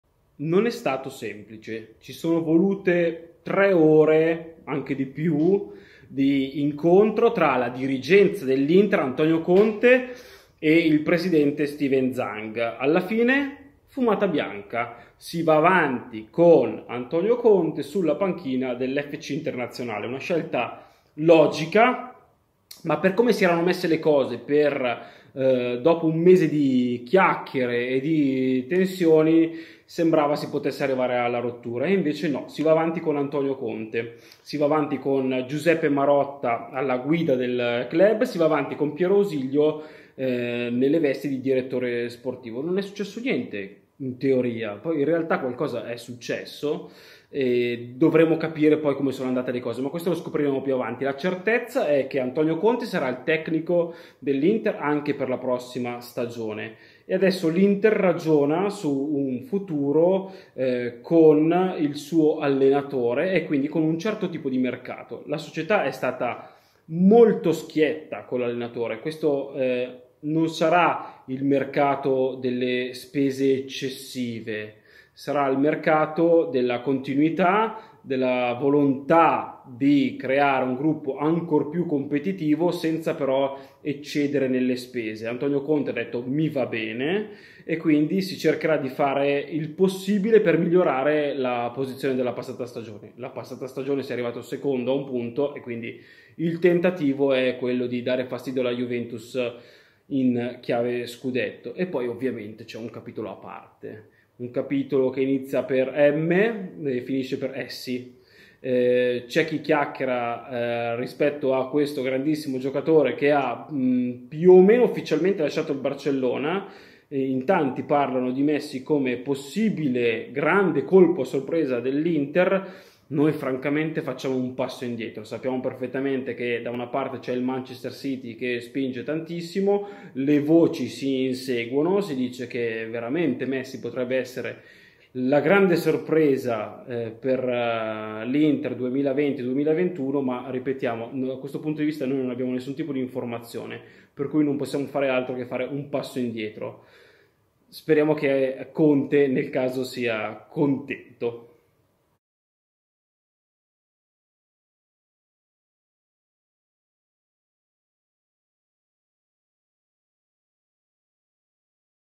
INTER, DALLA CONFERMA DI CONTE ALLE ACROBAZIE SU MESSI ⚽️⚫️🔵
I nerazzurri confermano l’attuale allenatore anche per la prossima annata: l’obiettivo ora è rinforzare la rosa, e qualcuno sogna un colpo di pura utopia... Leo Messi! 💰